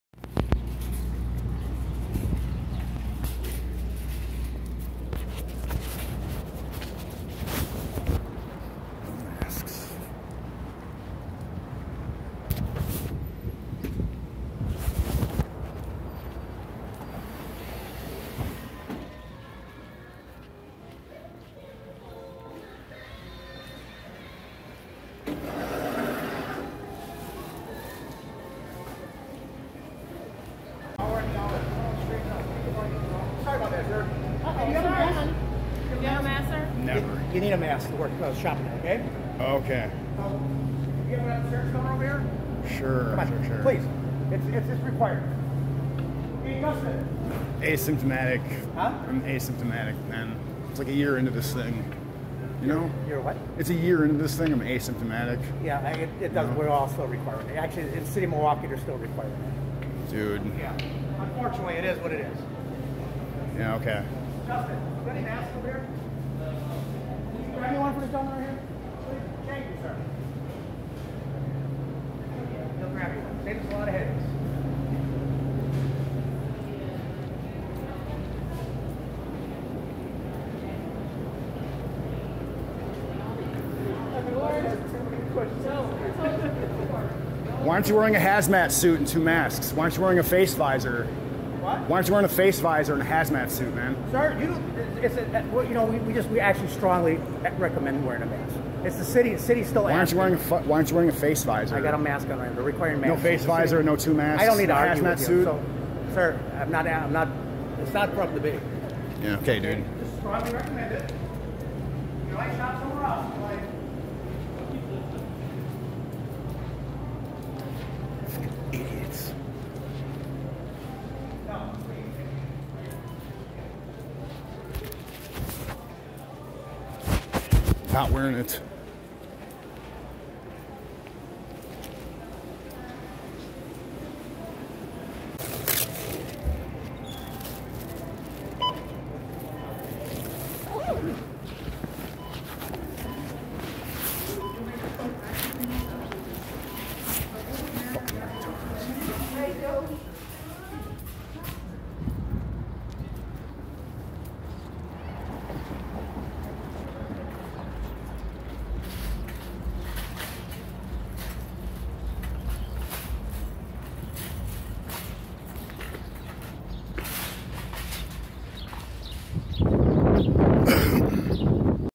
Refusing To Wear A Mask At The Grocery Store
Refusing To Wear A Mask At The Grocery Store. Look at this absolute power tripping maskie psycho. All it takes is to sarcastically brush these idiots off, and you too can live a mask free life! Ahhhhh! Breathe that fresh Spring air!
BTC:  16DfhbdZD2EcDC8Gv82t66dm1zE1xDUboG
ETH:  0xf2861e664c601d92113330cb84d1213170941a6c
LTC:  LZEZzxLdjzPUfUHJbXmvEhjduYQ5tn4FEs